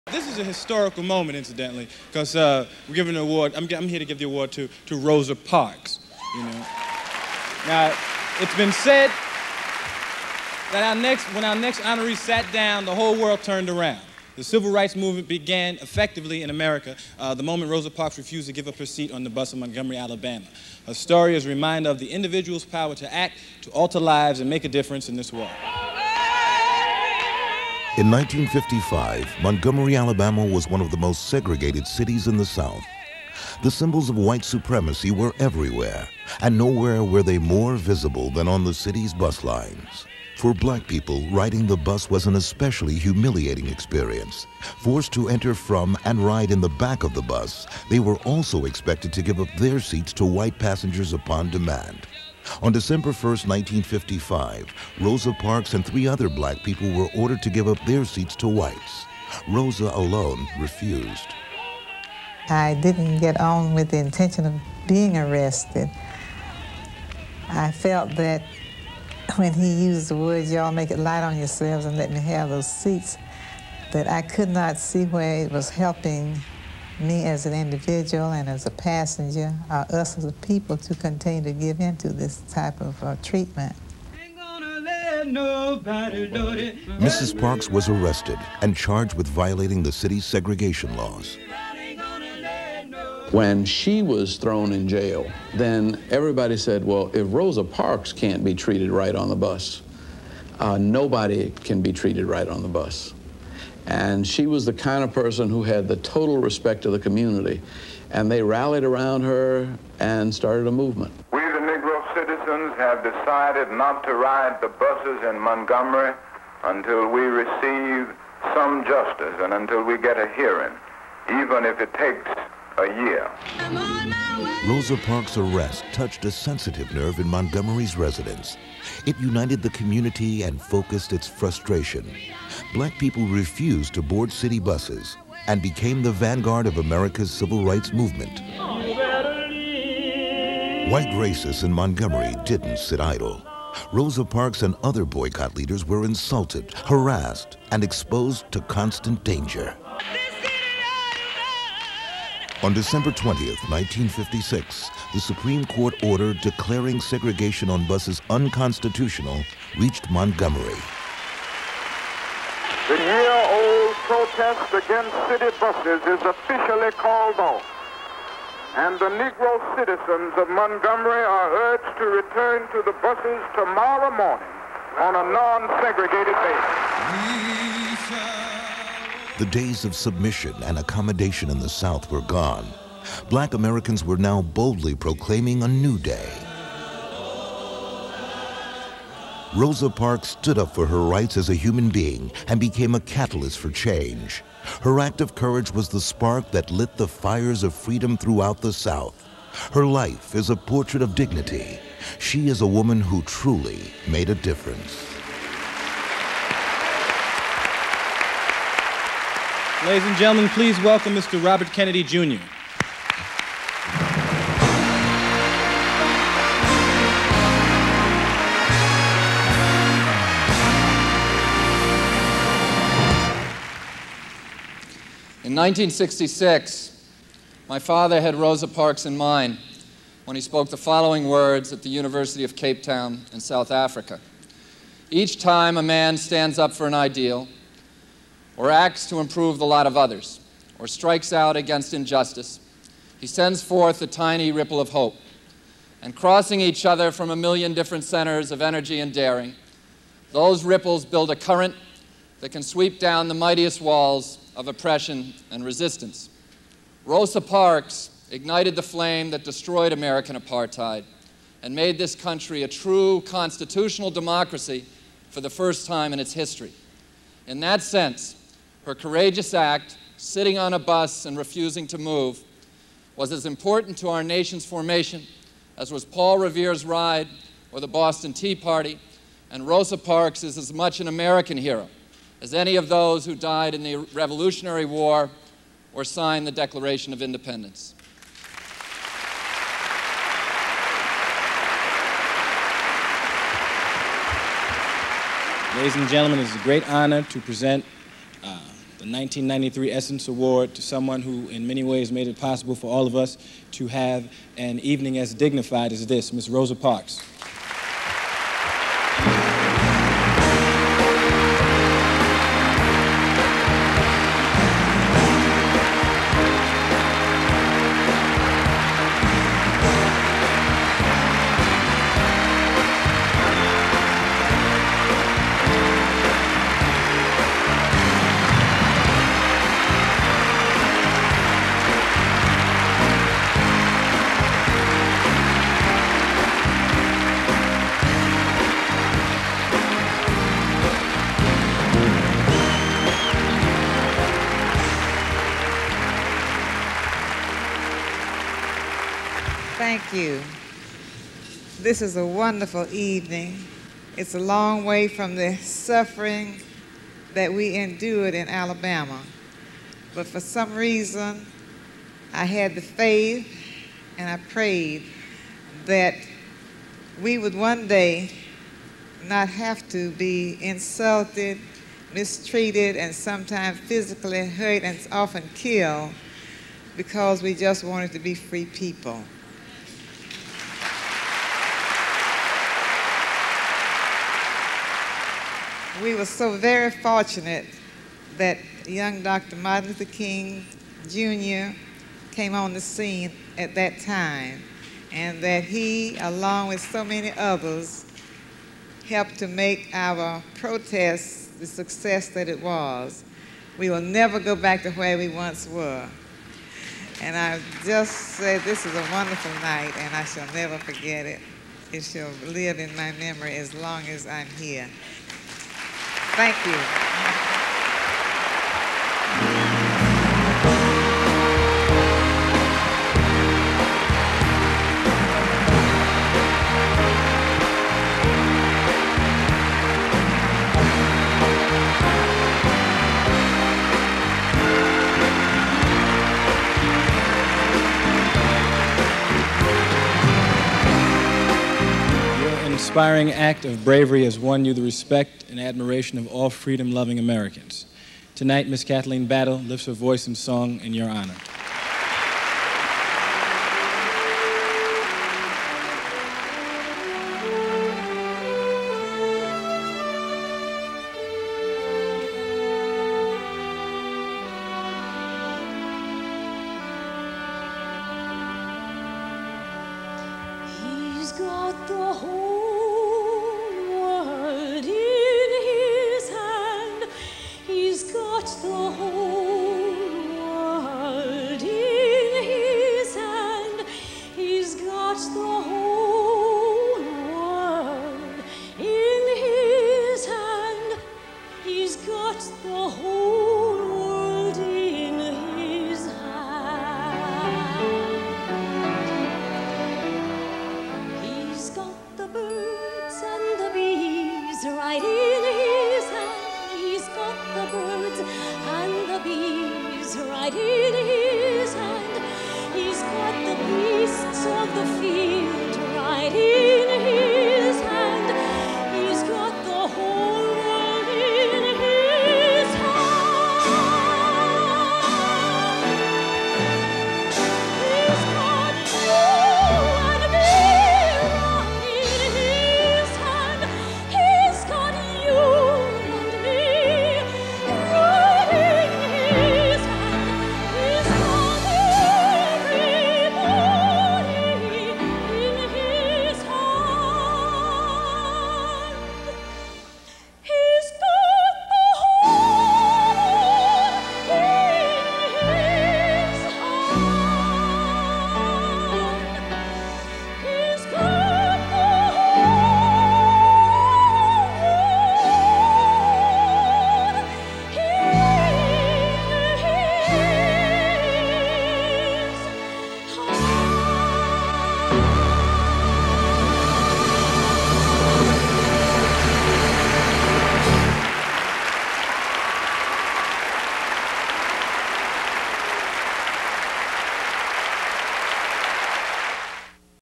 Rosa Parks receiving the 1993 Essence Award
Rosa Parks receiving the 1993 Essence Award.  She is introduced by Eddie Murphy, honored by Robert Kennedy, Jr. and then entertained by Kathleen Battle.  Elaine Steele, the co-founder of the Rosa & Raymond Parks Institute can also be seen in the footage sitting next to Mrs. Parks.